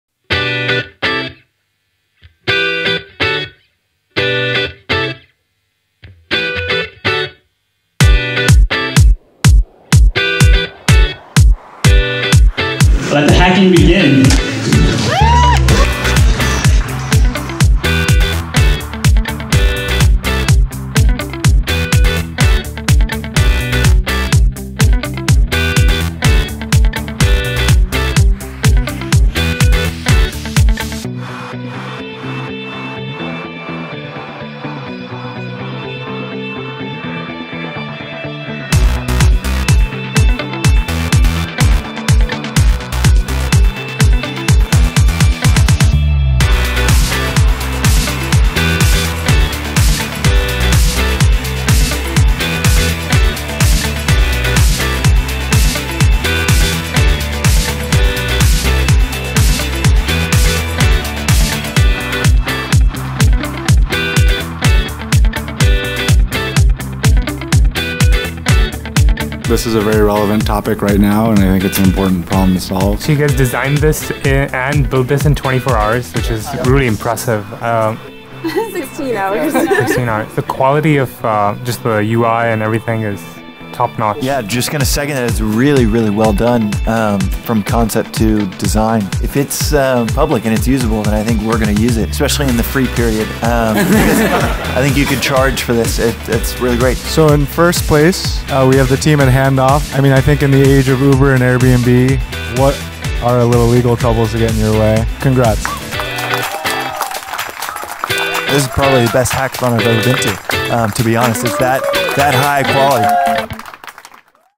LinkedIn Intern Hack Day 2015
LinkedIn’s 5th Annual Intern Hack Day attracted 220 interns from tech companies all over the Bay Area including Facebook, Twitter, and Google. Special guest judges included Tilt’s CEO- James Beshara, our Co-Founder of Pulse & Head of LinkedIn India- Akshay Kothari, and Hotspots founder- Matt Huang. The $10,000 grand prize went to “Handoff”, a Google Chrome extension that allows groups of friends to easily and securely share different accounts with each other without revealing any login credentials. Supported services include Netflix, HBO, The New York Times, and NFL.